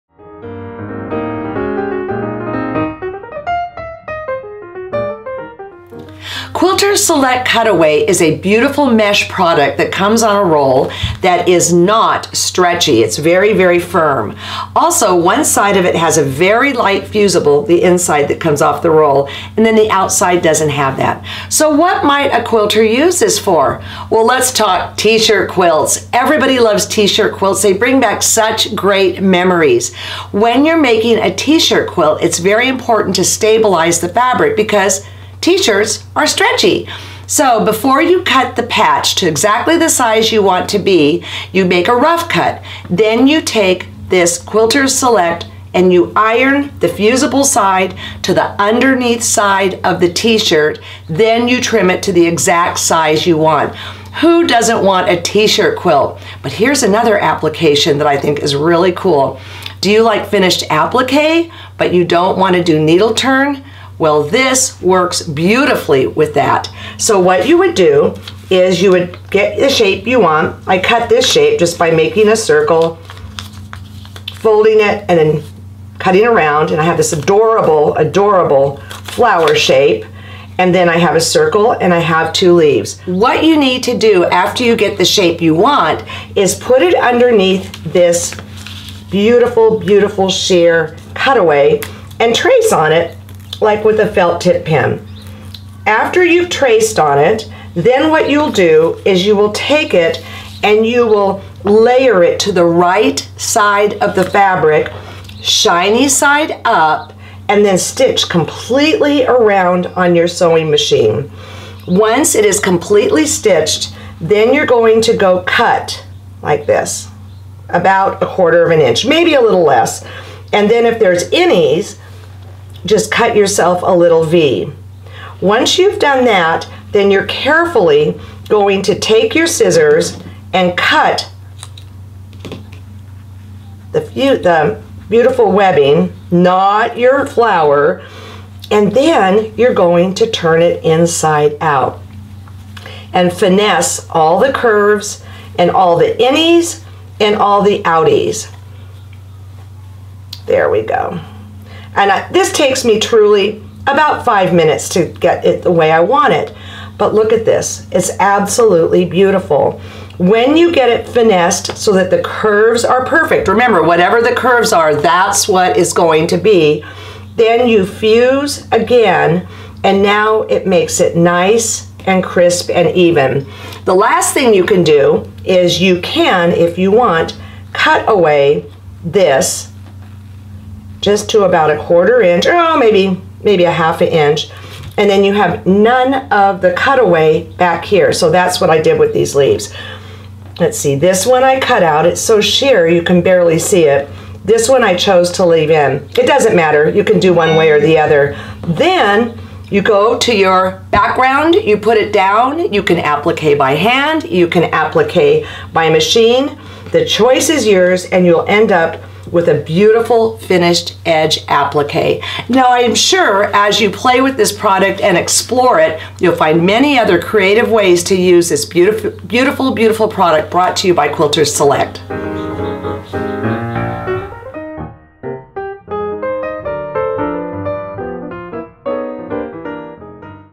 Quilters Select: What Is Select Cutaway?
Want an appliqué shortcut? Need to make a T-shirt quilt? Select Cutaway prevents fabrics from stretching or distorting.